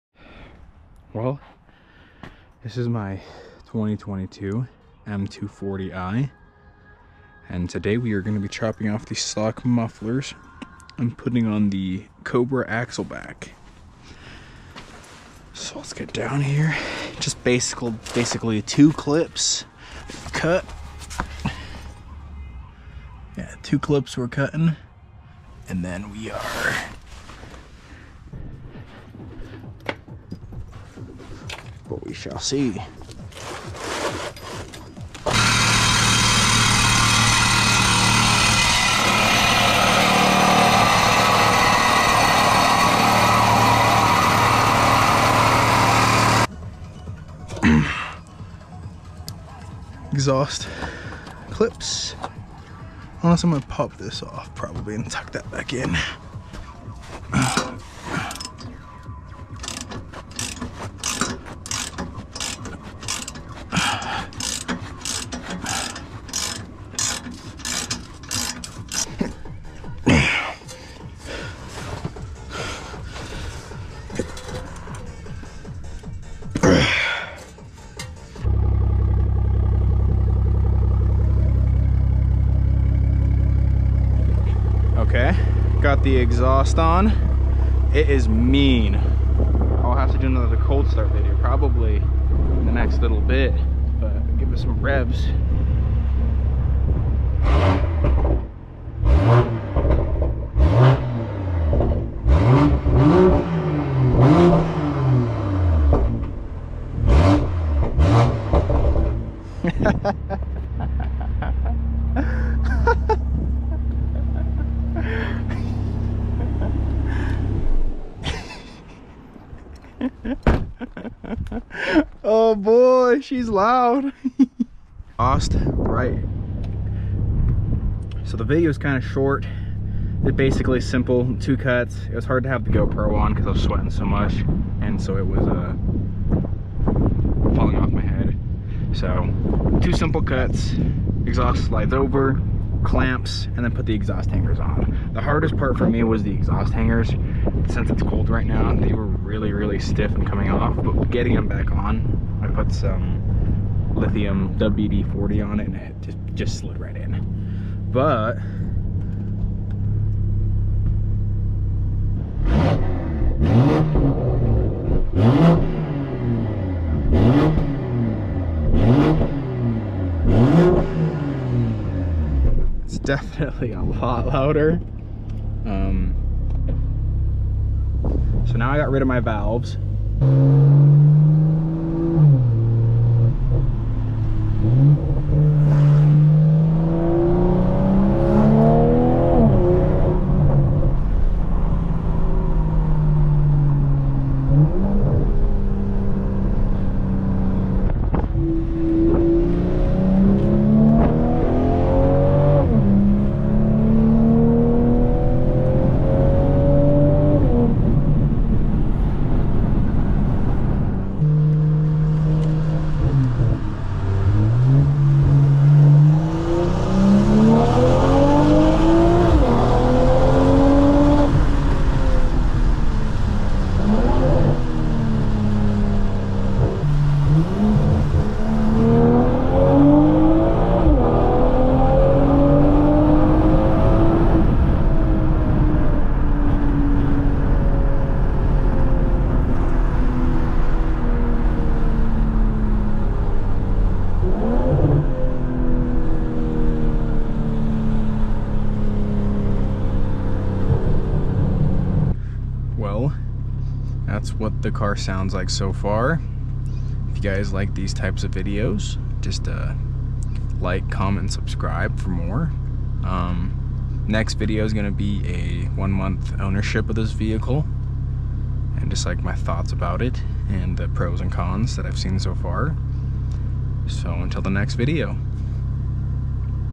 2022 BMW M240i Cobra Axle Back Install and POV Drive w Sounds!
The car now has the AA Highflow catted downpipe and the Cobra Axle Back! SHE IS SO MEAN! GET THE AXLEBACK